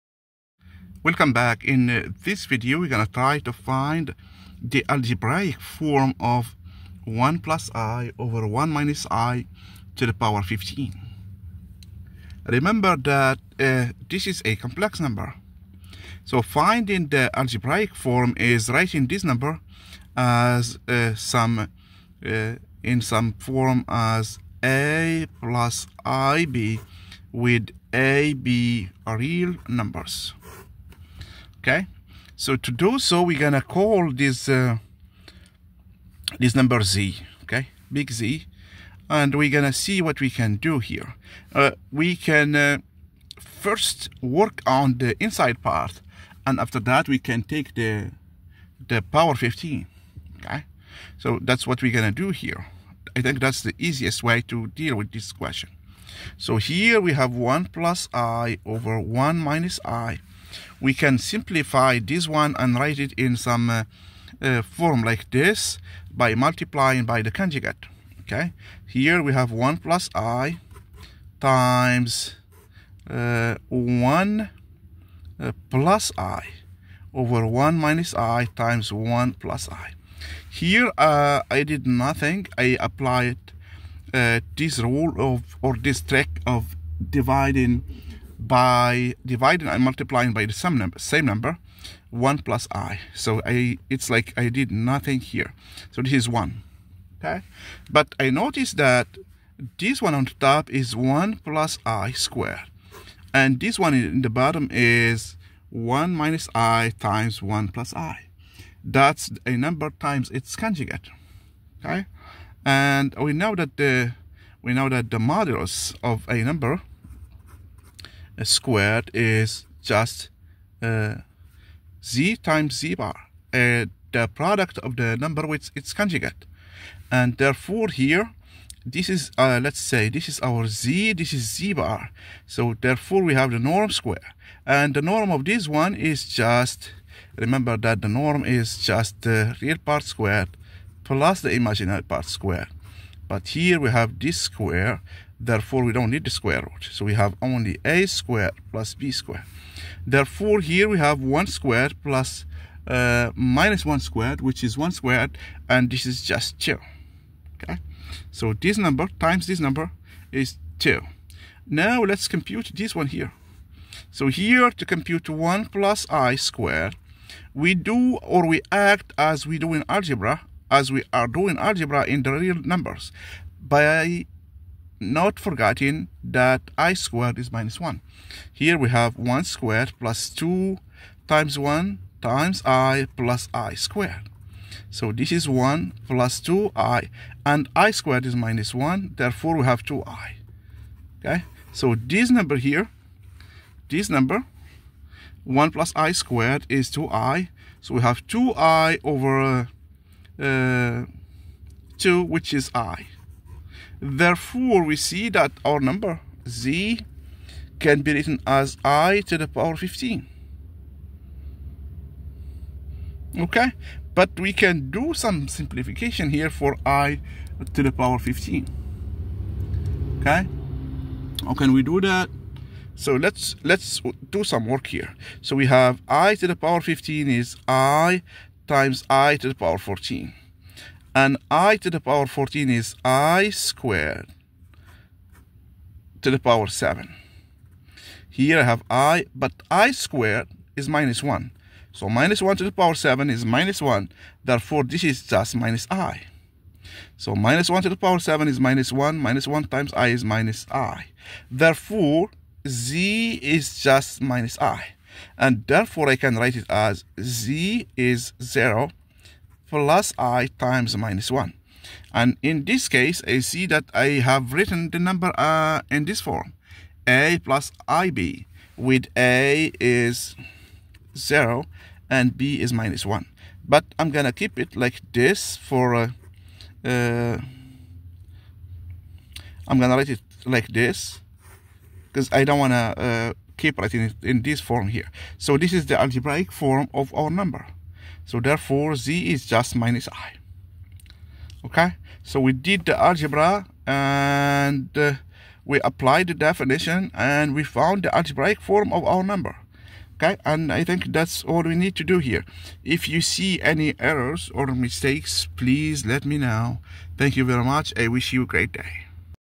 The algebraic form of a complex number . Working one example in details.
The algebraic form of a complex number . Working one example in details. maths olympiad calculus education python chemistry howtosolveolympiadmathproblem mit physics ap calculus complex analysis.